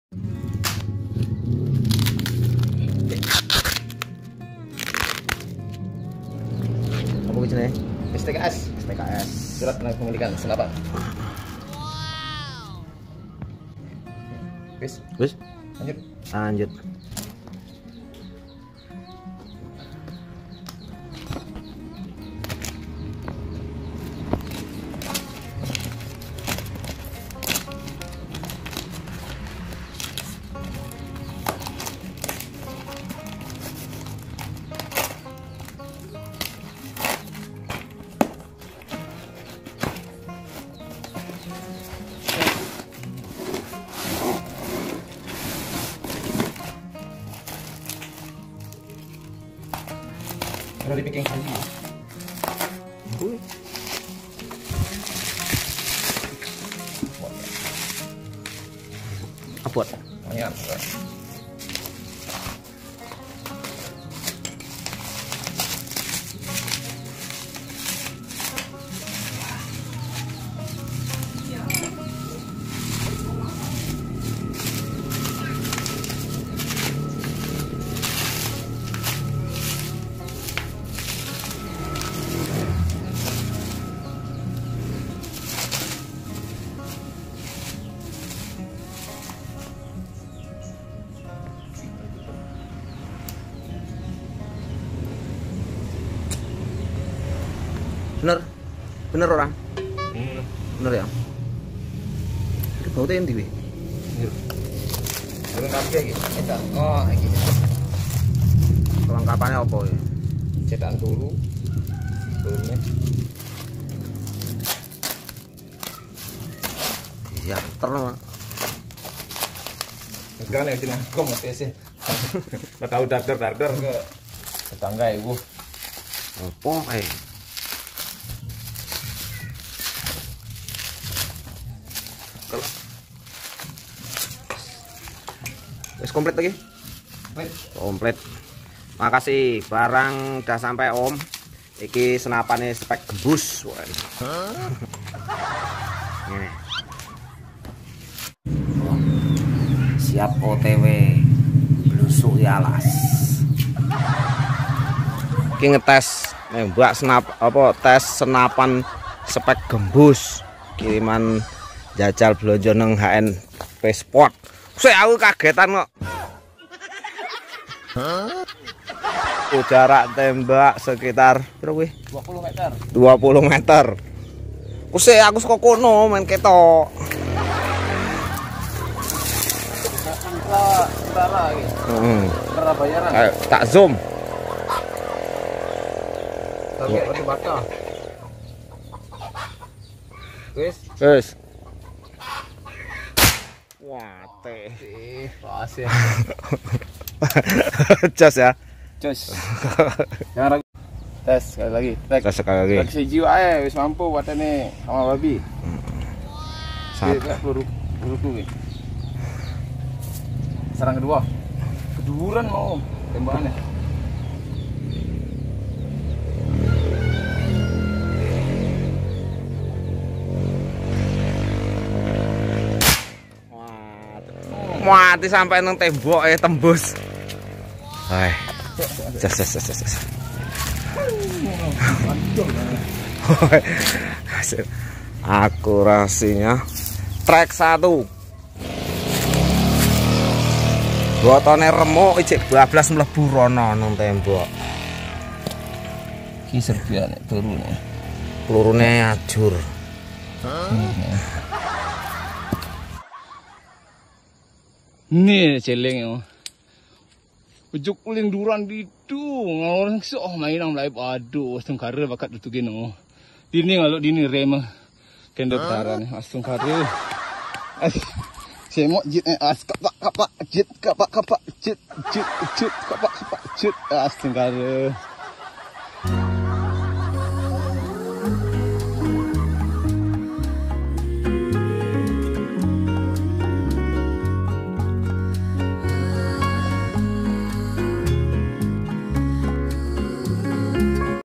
PCP jawa Spek Gembus babon Josss
Unboxing pcp jawa basmi hama babi , kualitas josss jitu amanah hnp sport